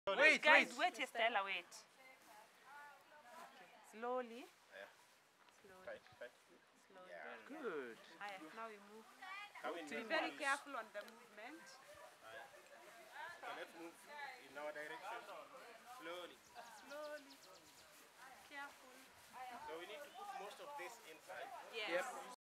Team Building Game 3 Challenge
This Game Requires coordination among team members , Team Work ,Communication and overall team leader.